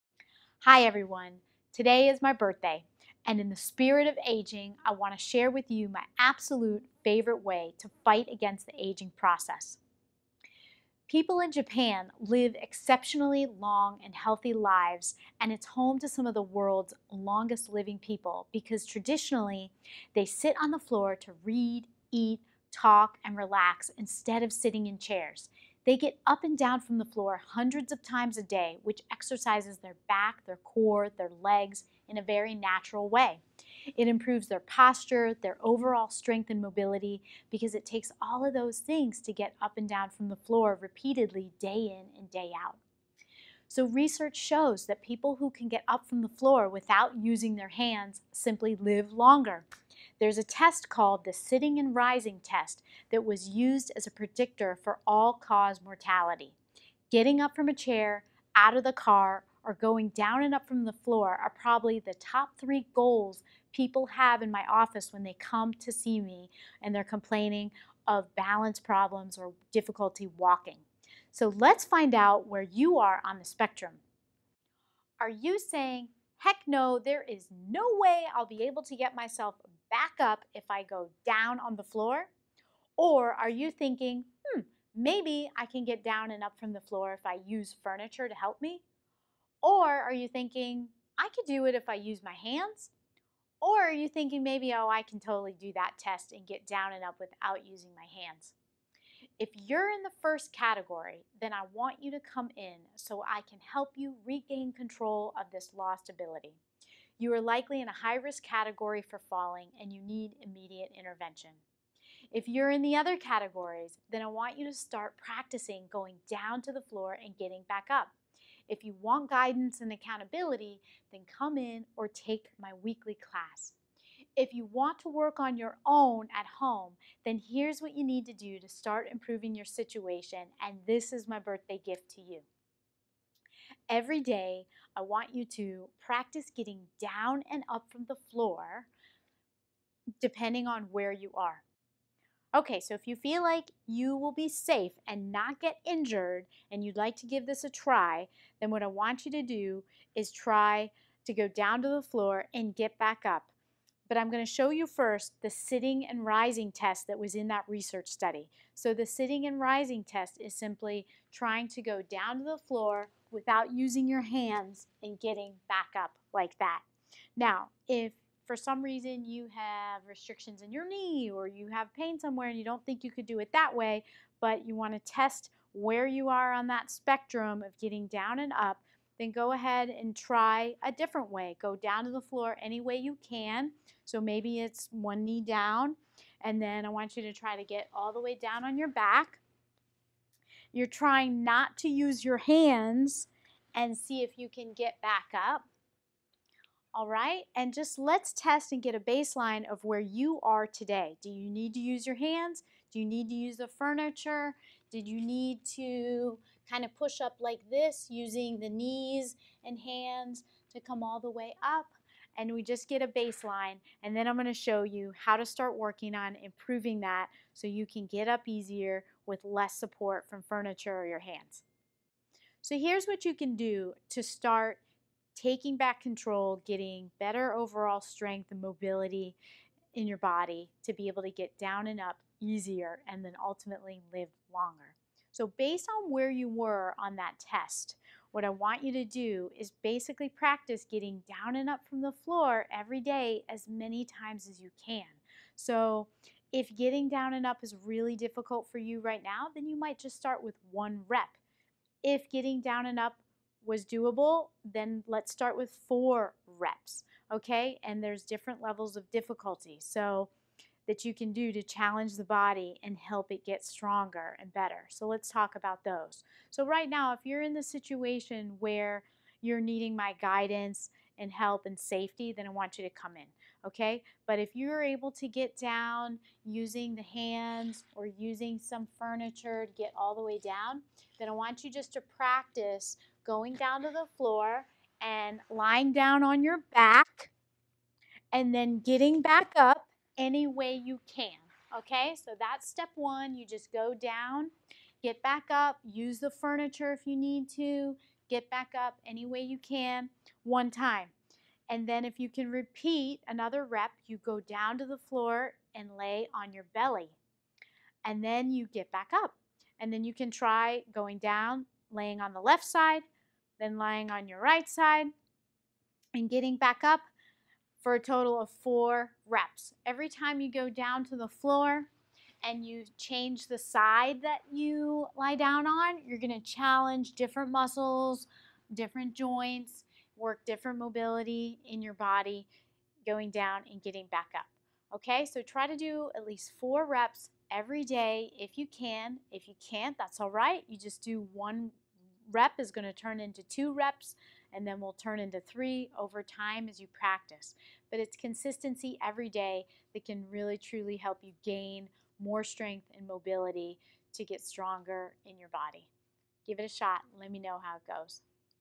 Sitting Rising Test
There have been a few studies showing the correlation of being able to get up from the floor without using hands and living longer.

Only perform this test if you are able to and in a safe environment.